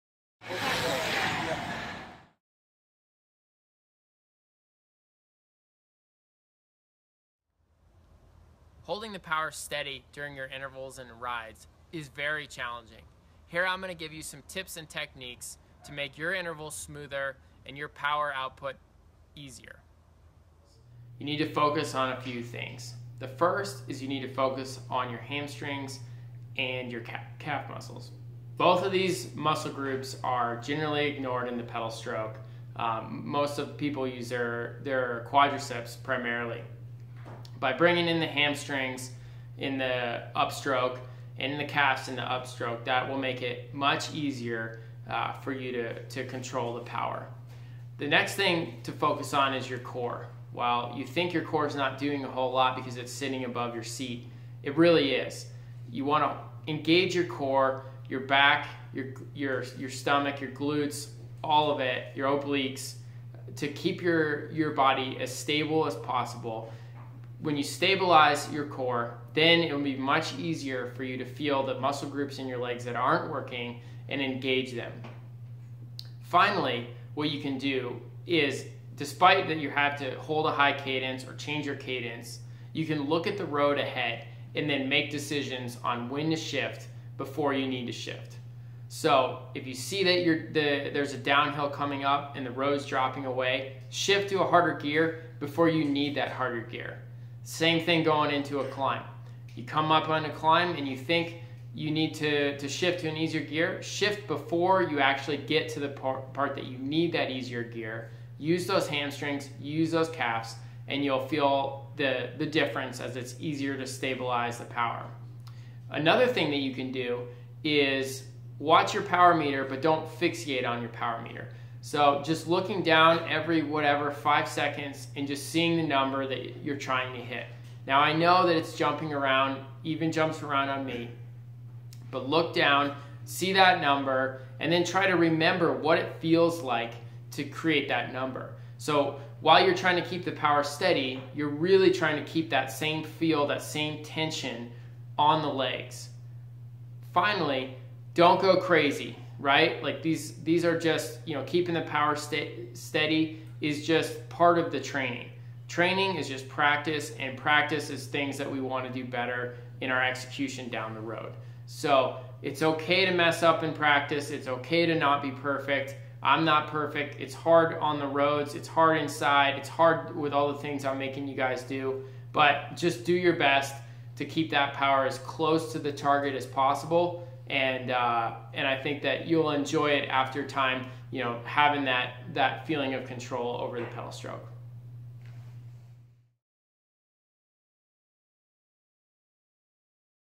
Holding power steady
Coach Tom Danielson shows us how to hold the power steady during your intervals and out on the road.